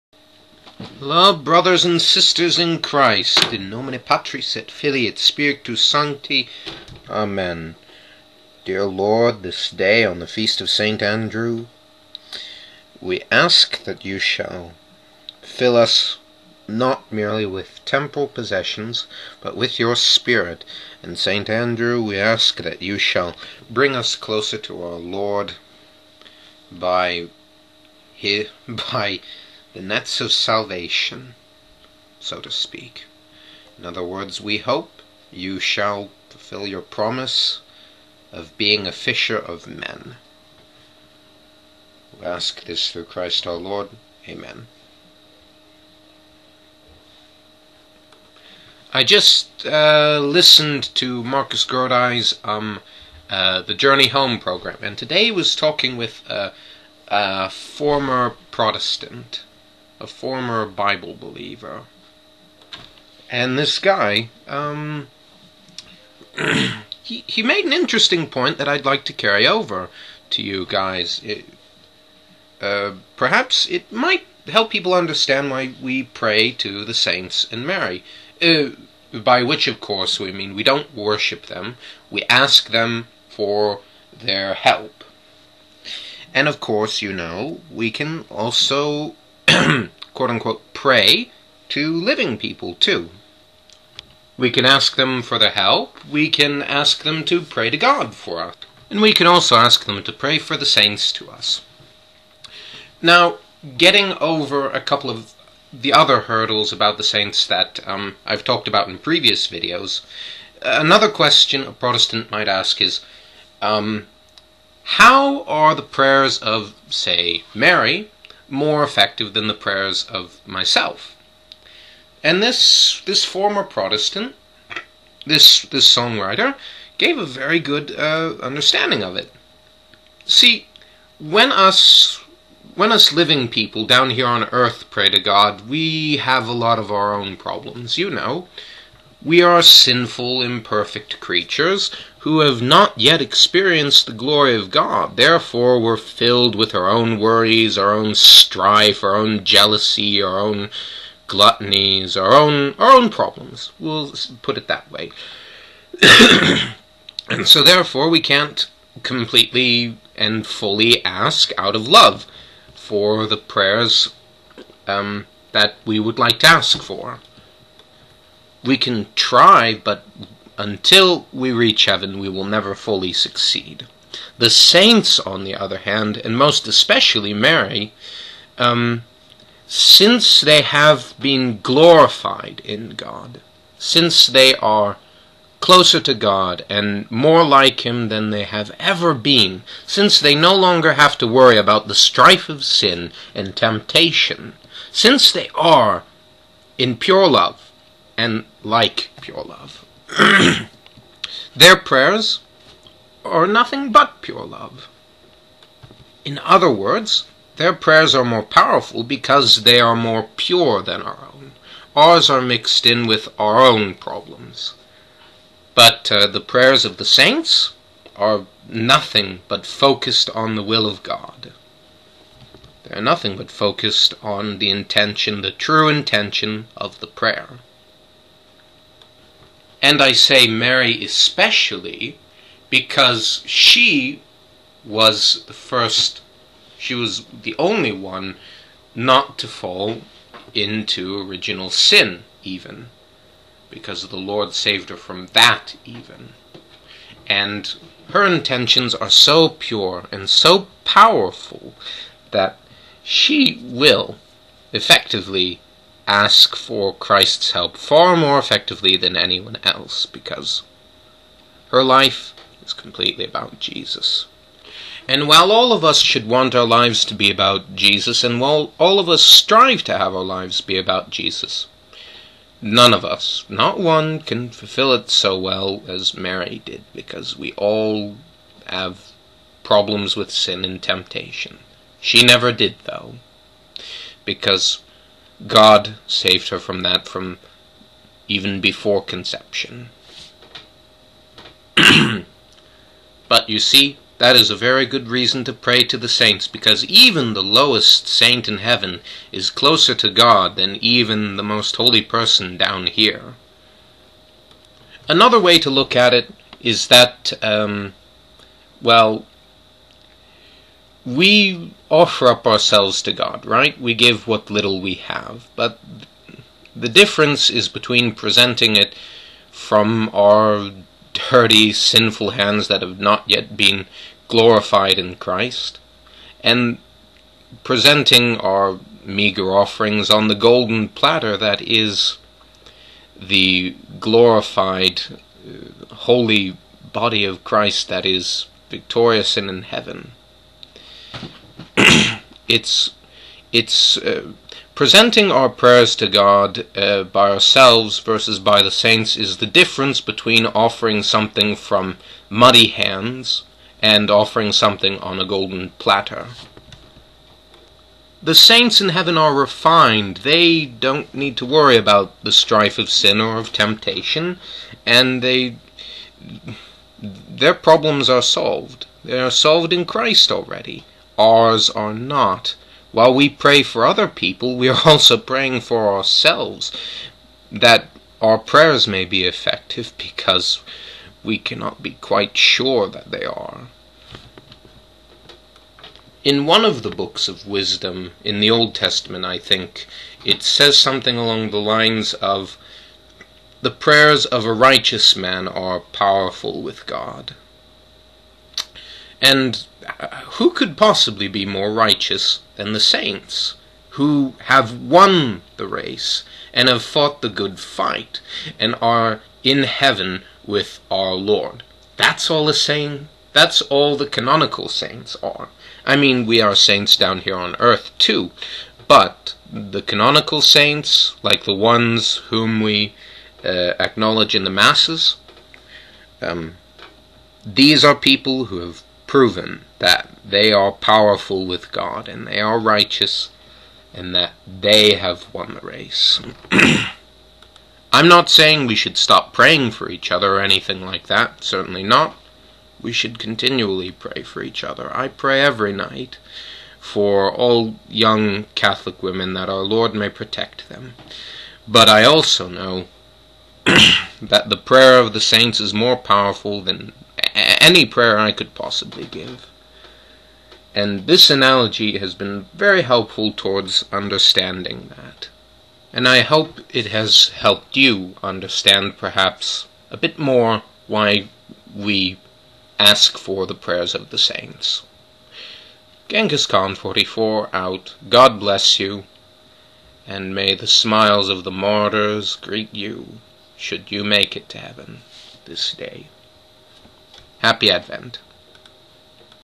What Makes the Prayers of the Saints Powerful with God
I think our prayers are not nearly as potent as those of the Saints in Heaven. And here is why.

Does that mean we should not pray for each other? Of course not.